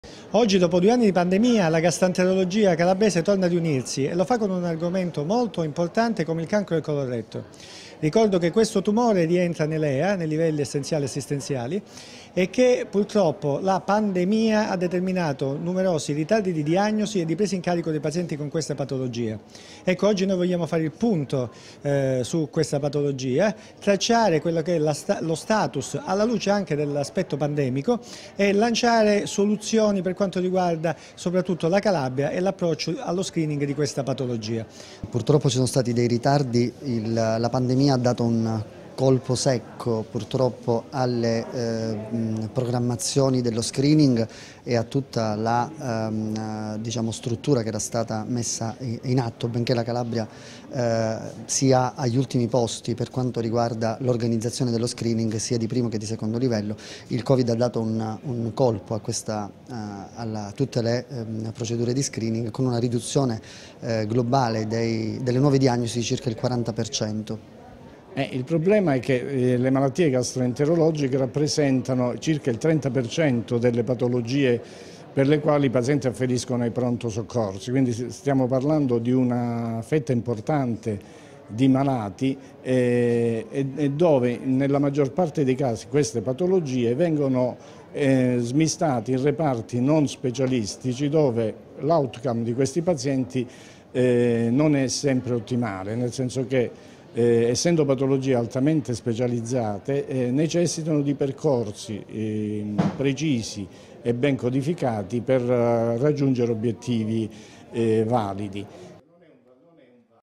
Catanzaro: il tumore del colon retto, dallo screening alla terapia
Congresso a Catanzaro "Il tumore del Colon-Retto, dallo screening alla terapia"